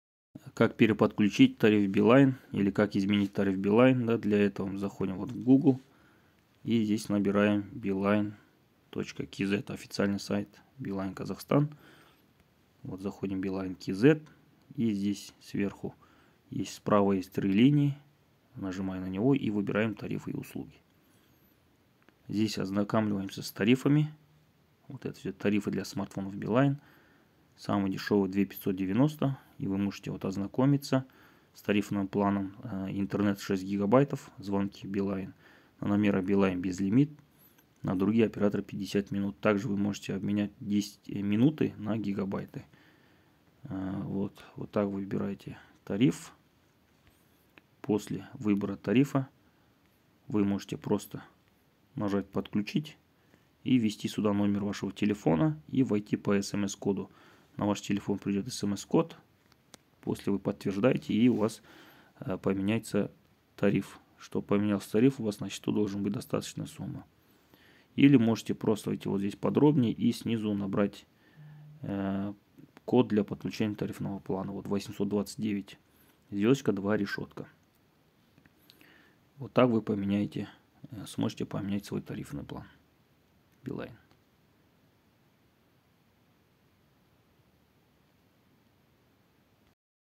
Как изменить тариф Билайн
Как переподключить тариф Билайн Казахстан или как изменить тариф Билайн

Для этого заходим в официальный сайт beeline.kz

и ознакамливаемся с тарифами, выбираем нужный для Вас тариф и подключяем тариф Билайн